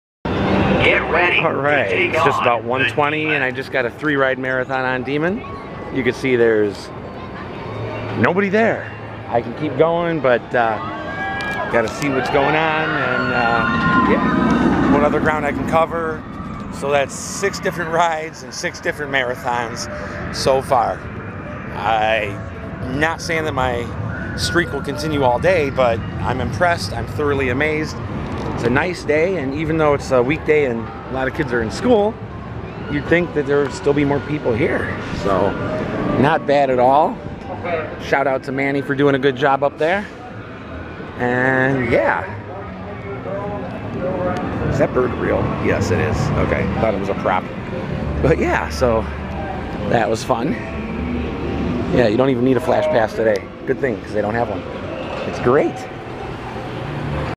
Demon Marathon Review Six Flags Great America 5-15-19
Another coaster, and yet another marathon! Demon had no line and was an enjoyable 3 in a row!